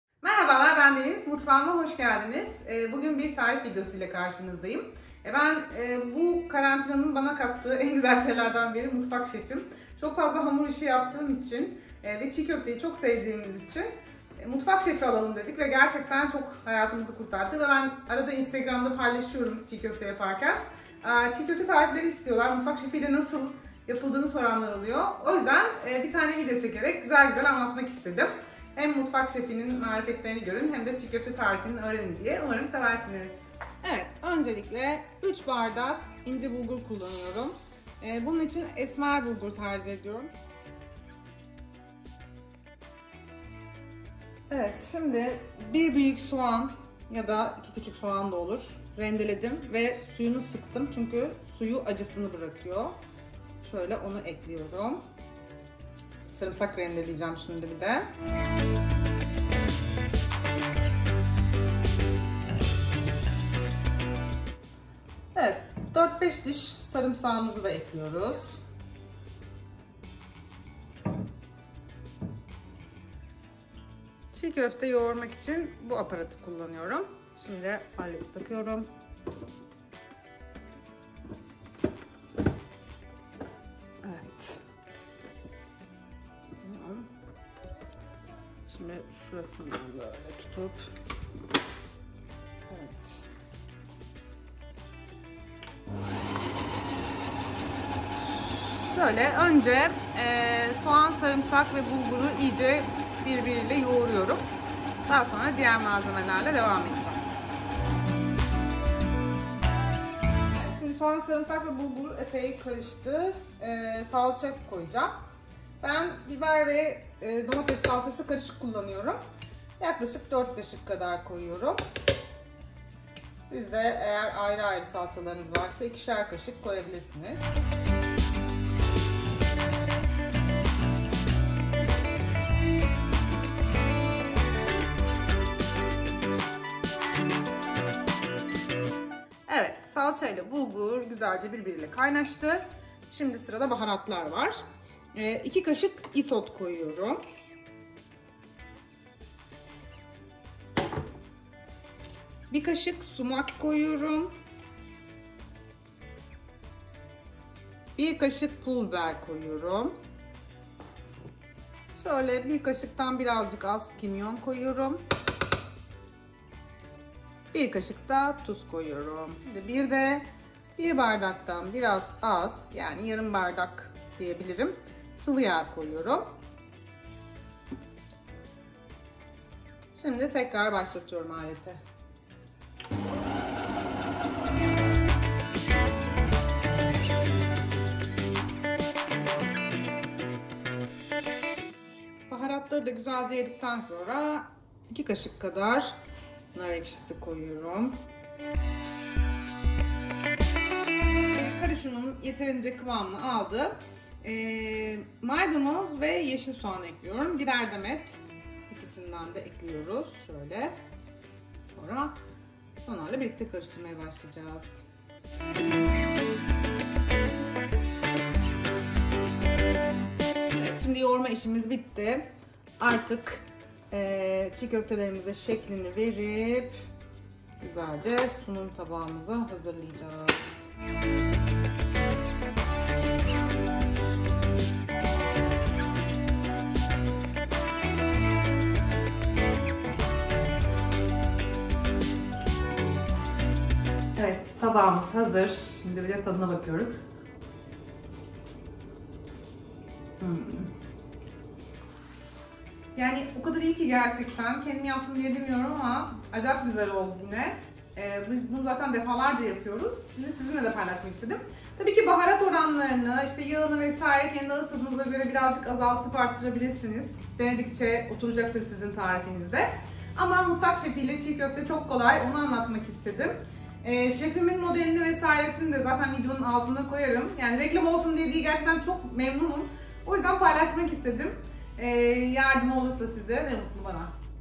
Mutfak Şefi ile Tam Kıvamında Çiğ Köfte
Kenwood mutfak şefi ile tam kıvamında leziz ve pratik çiğ köfte tarifi.